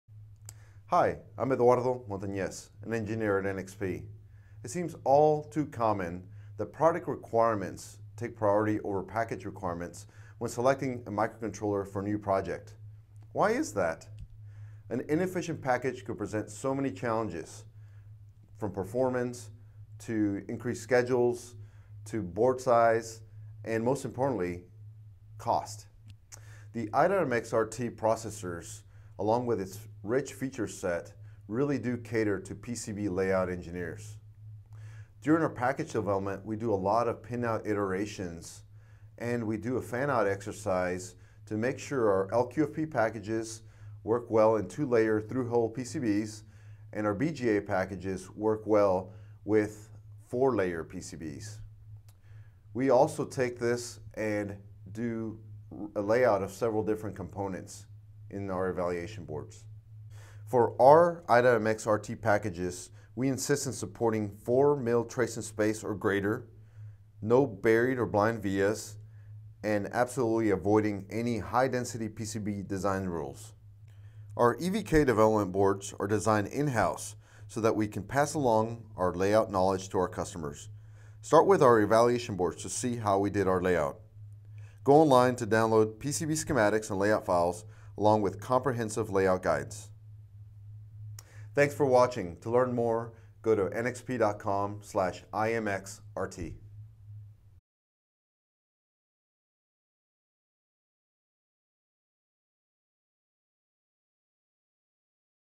PCB design with i MX RT
Learn how the i.MX RT crossover processors can significantly simplify PCB design for engineers.